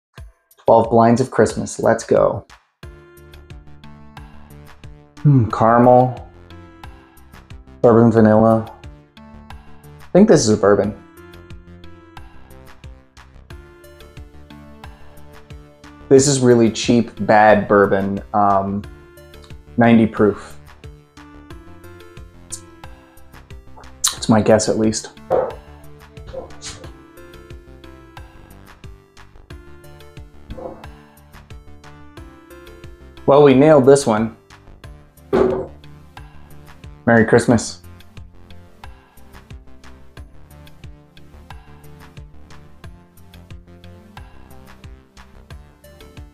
How bad are Total Wine's Own Brands?
Rules: My wife pours me a blind sample or a blind bottle from my shelf as long as I haven't reviewed it before. I must guess three from the following categories: age, proof, type, finish, or try and snipe a bottle guess. Age is +/- 1 year and proof is +/- 5.

Merch is finally available! This stuff is perfect for any Whiskey lover, not just YDB Stans. Check it out: